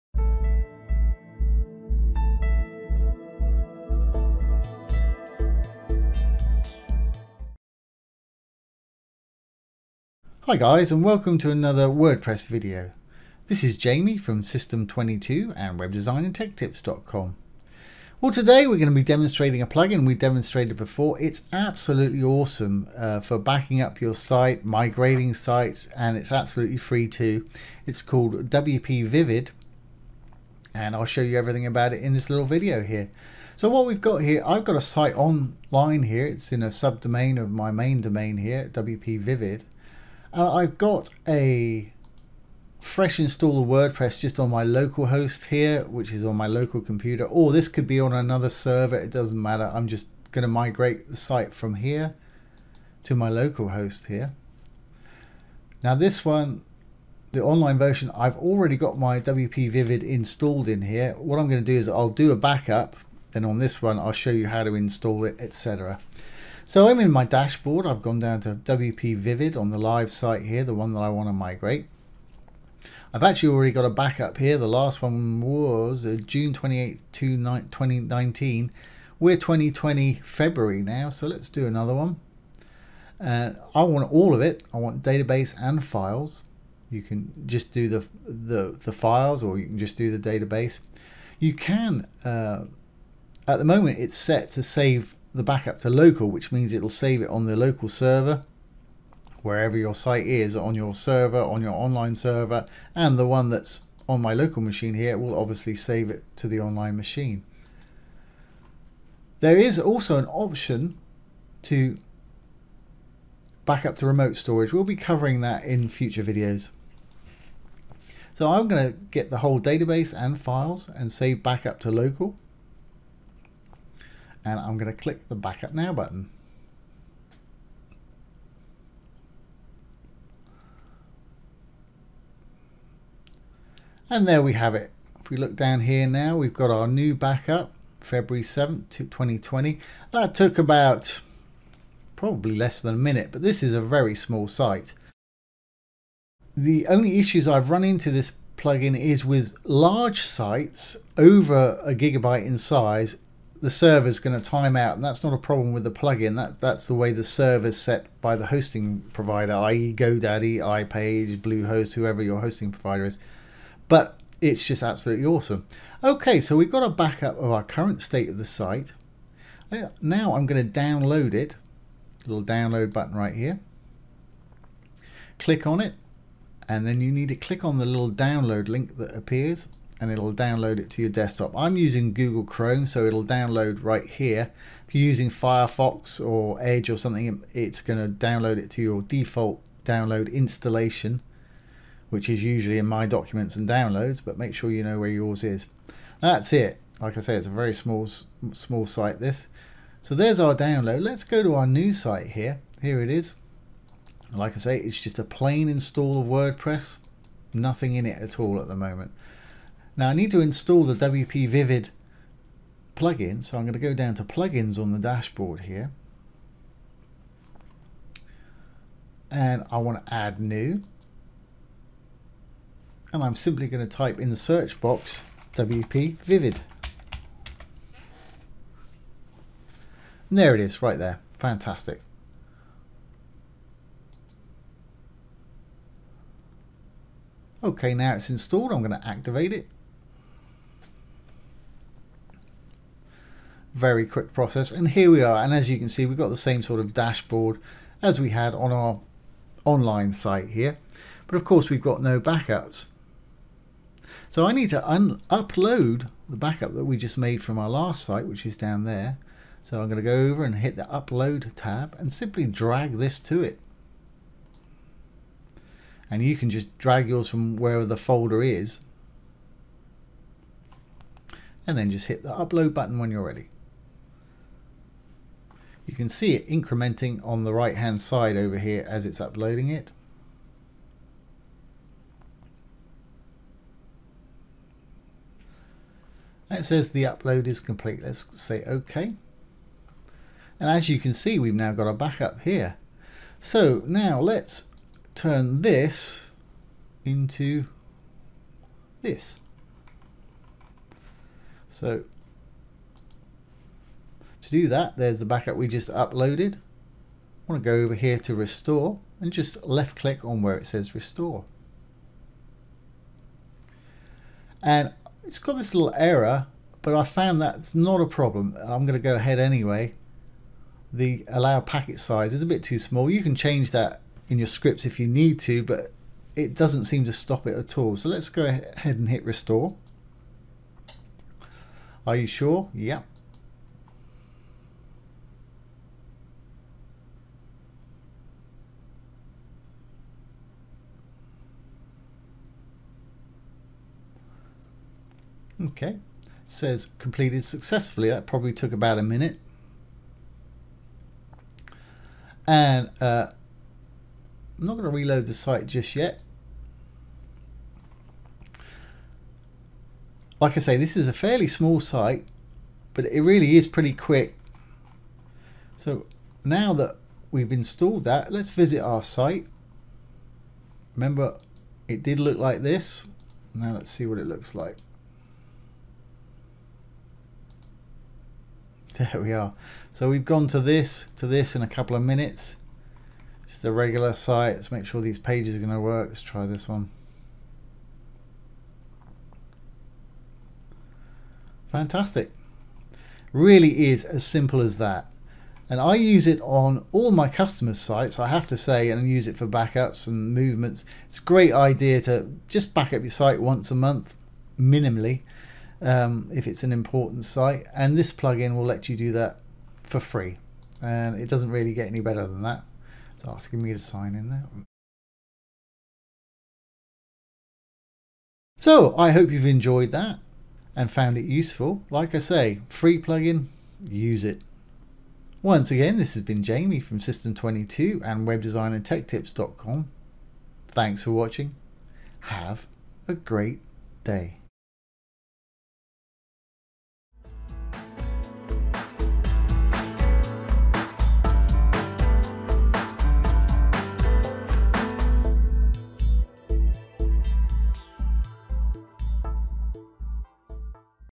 Wordpress Backup And Migrate Site With WPvivid Plugin
Wordpress Backup And Migrate Site With WPvivid Plugin. In this video check out how easy it is to backup move and restore a WordPress website with WPvivid plugin. WPvivid Backup Plugin is the only free all-in-one backup, restore and migration WordPress plugin. Clone or copy WordPress sites then move or migrate them to a new host (new domain). Off-site backup schedules, transfer backups to leading remote storage. All in one.